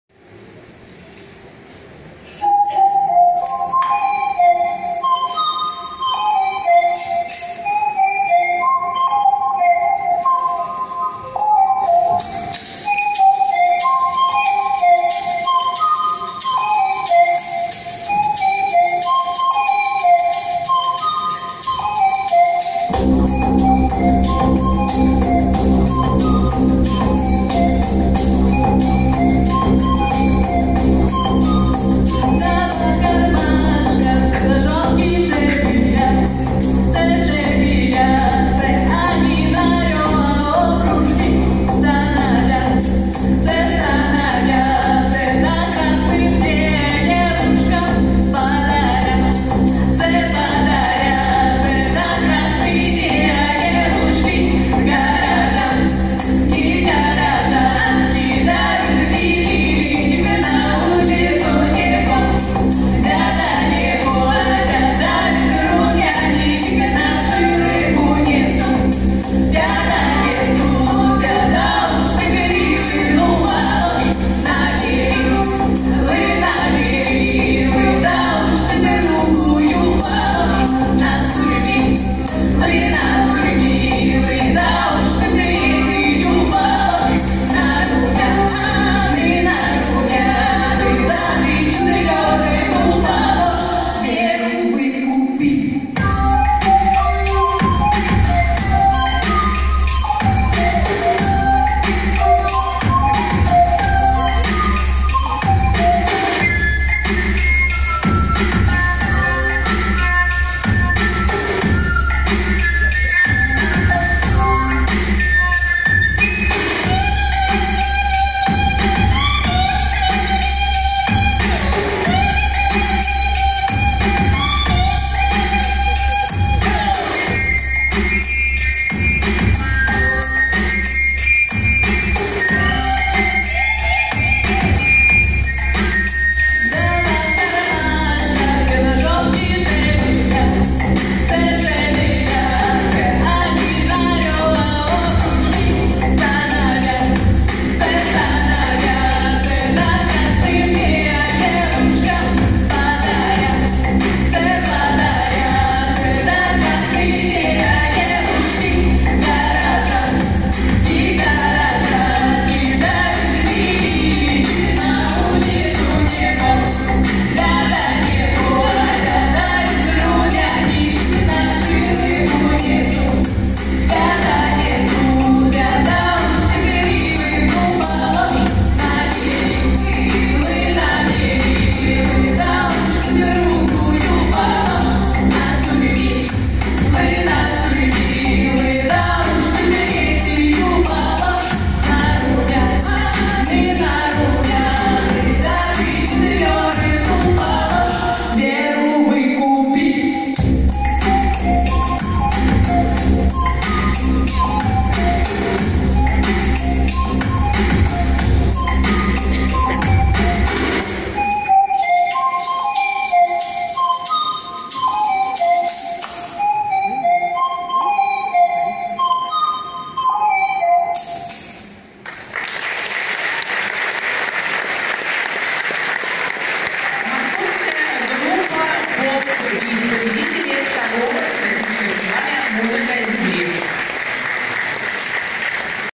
Музыка Земли 12.06.2017. Открытие. Группа FolkBeat, Москва
III Всероссийский фестиваль-конкурс "Музыка Земли", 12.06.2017. Открытие. Концерт в Российской академии музыки имени Гнесиных (РАМ)
Группа FolkBeat, Москва